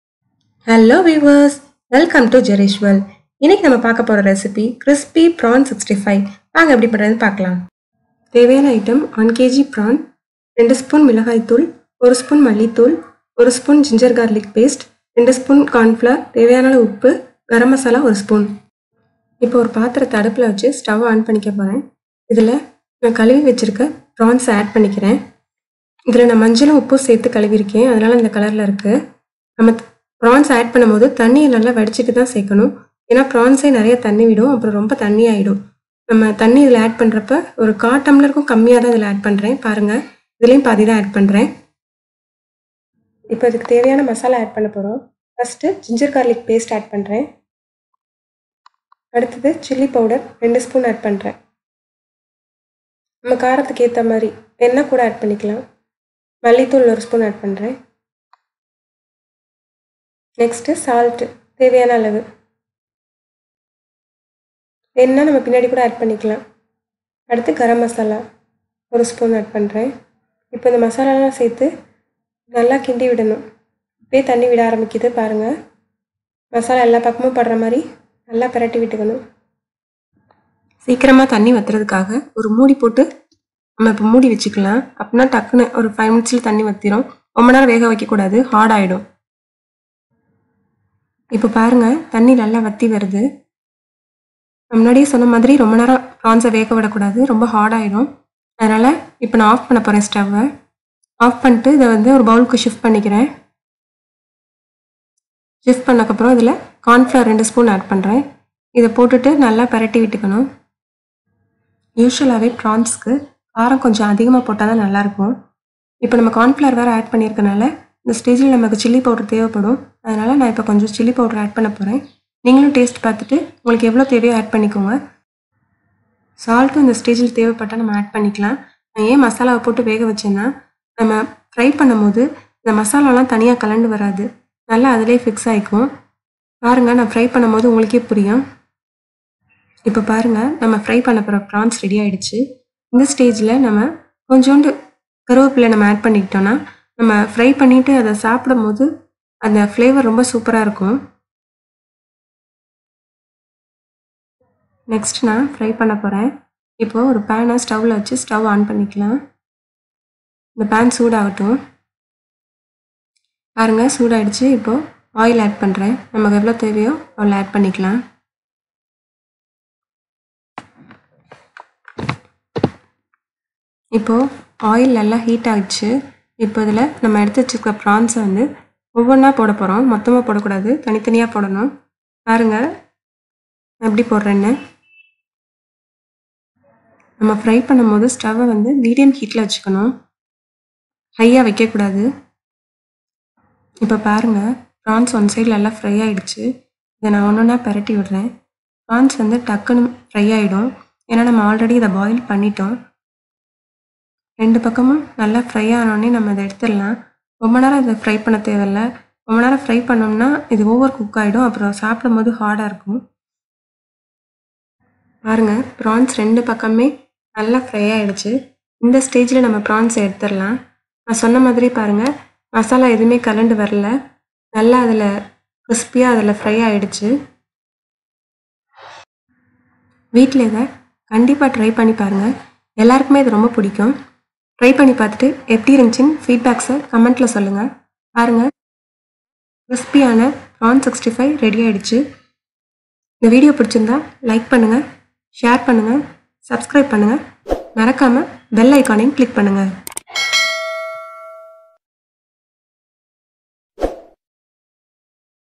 PRAWN 65 IN TAMIL | PRAWNS FRY | இறால் 65 செய்முறை
CRISPY PRAWN 65 RECIPE TAMIL | இறால் 65 செய்முறை

In this video, we will see how to make pepper prawns at home. Prawns fry is a frequently ordered side dish recipe in the restaurant. It is very easy to prepare and it tastes awesome. It can be served as an appetizer or as an accompaniment to ghee rice. It is a treat for all prawns lovers.  Friends please do try this easy prawns fry at home and amaze your friends and family with this easy and delicious side dish. Also please post your feedback in the comments below.